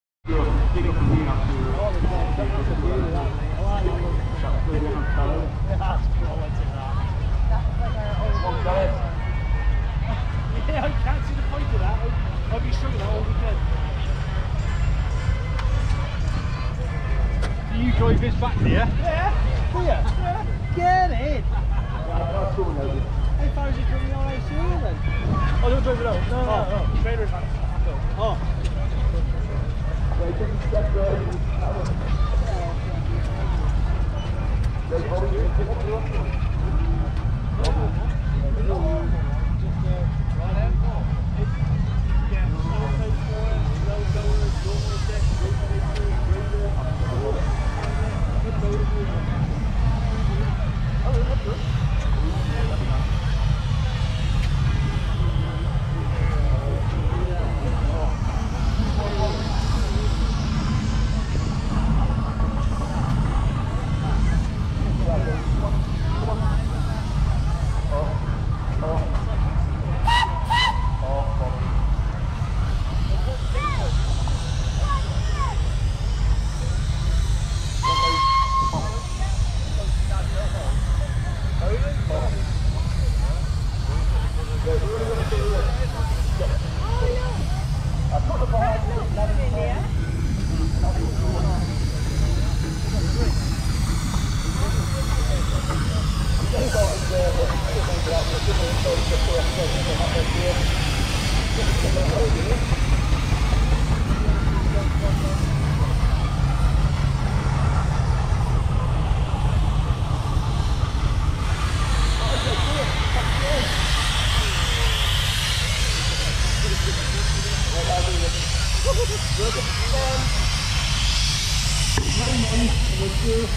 1942 John Deere BN 2.4 Litre 2-Cyl all fuel Trike Tractor (17 HP)
1942 John Deere BN 2.4 Litre 2-Cyl all fuel Trike Tractor (17 HP)

Welland Steam & Country Rally - Welland, Worcestershire, England, United Kingdom - Friday 29th Saturday 30th & Sunday 31st July 2022